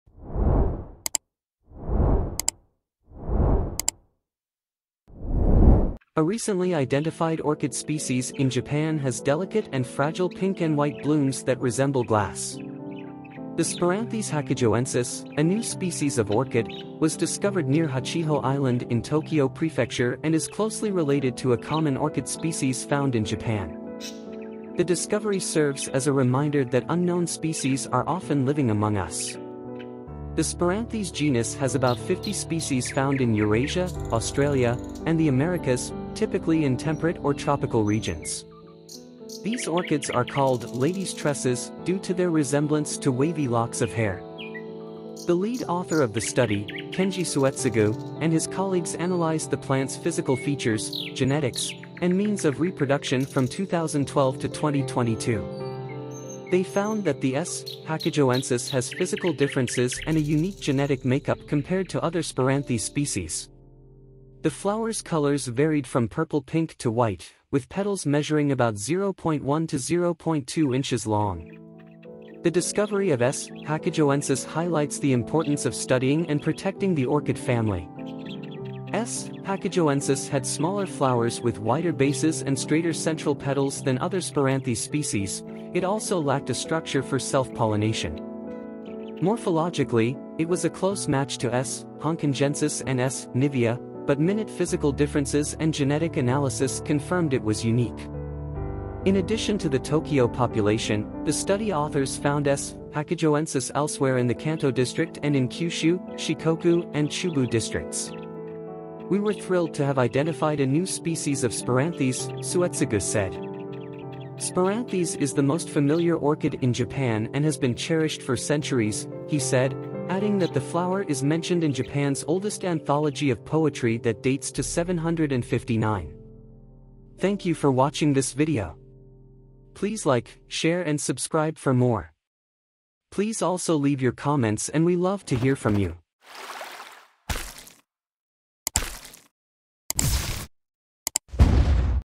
A new species of orchid has been discovered in Japan - 2023 New Discovery
Share this wonderful news to the world! These orchids are called “ladies’ tresses” due to their resemblance to wavy locks of hair.

Source:
The rosy pink petals of Spiranthes hachijoensis.
 Photograph: Masayuki Ishibashi/Kobe University